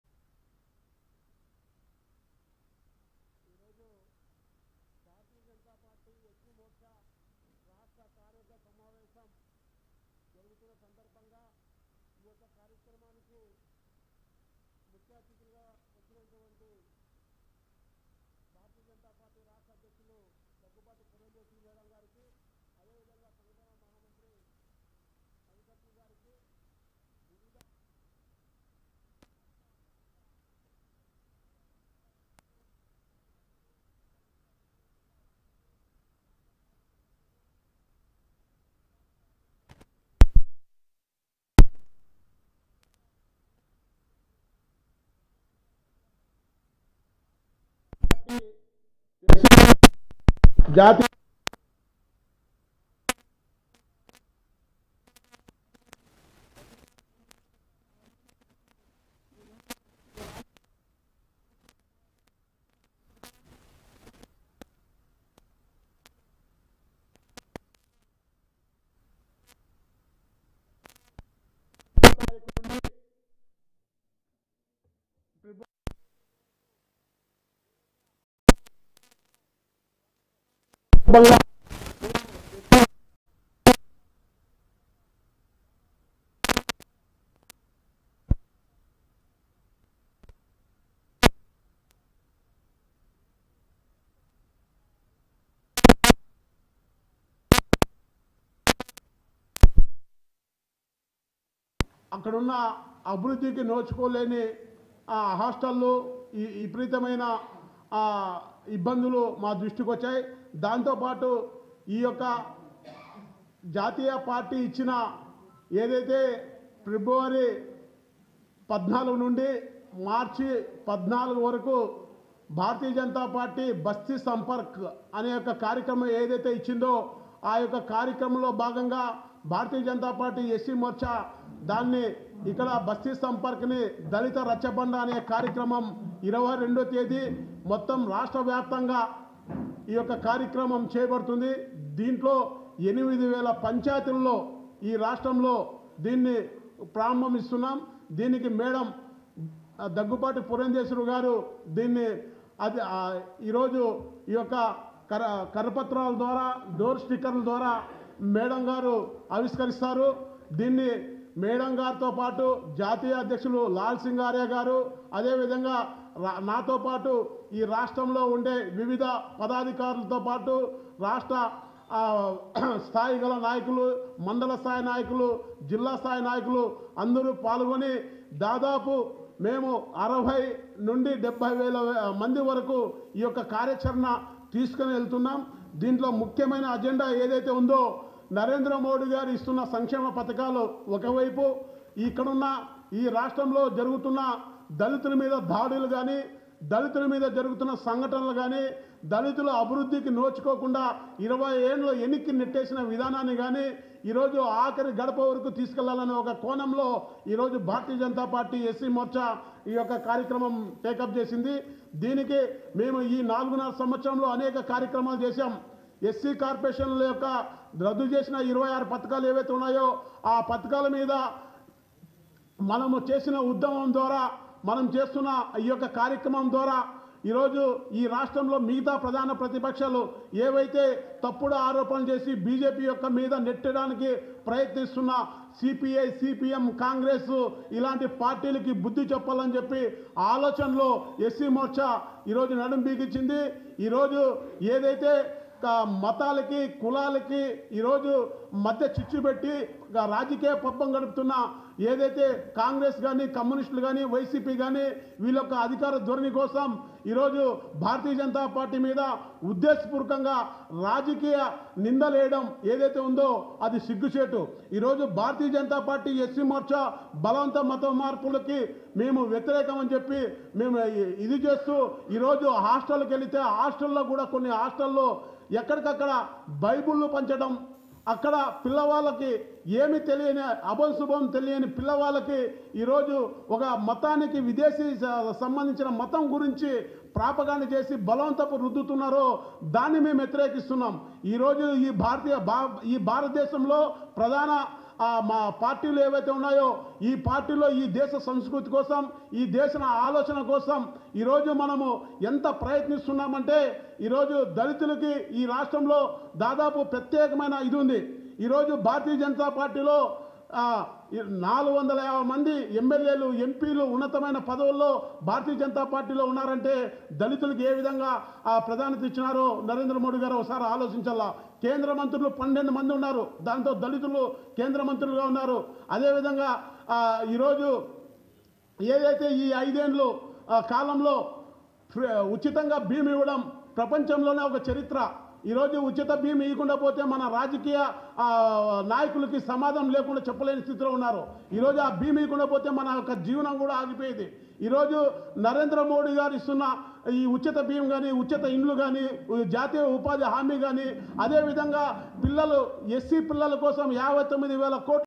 BJP Yuva Morcha Secretary Sri Gudise Devanand Press Meet BJPAP
మతాలకు - కులాలకు మత మార్పు కు బీజేపీ వ్యతిరేకం..
BJP Yuva Morcha Secretary Sri Gudise Devanand Press Meet BJPAP | AP Politics